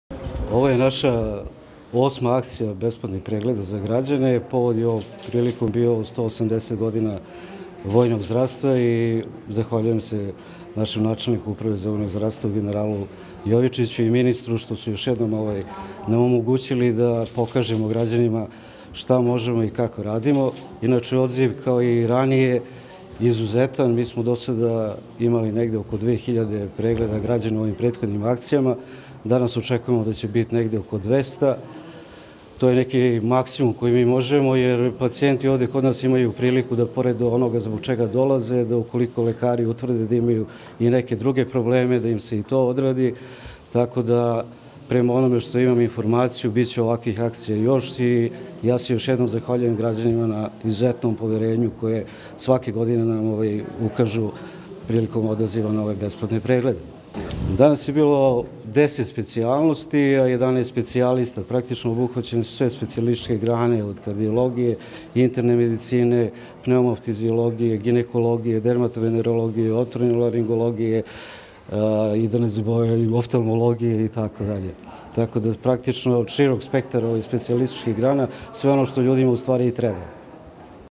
Izjava pukovnika Staletovića povodom besplatnih lekarskih pregleda u VMC Karaburma
Ministar odbrane Aleksandar Vulin posetio je Vojnomedicinski centar Karaburma u kome su danas, u okviru akcije Ministarstva odbrane povodom obeležavanja predstojećeg Dana Sanitetske službe i 180 godina srpskog vojnog saniteta, organizovani besplatni lekarski pregledi za sve građane.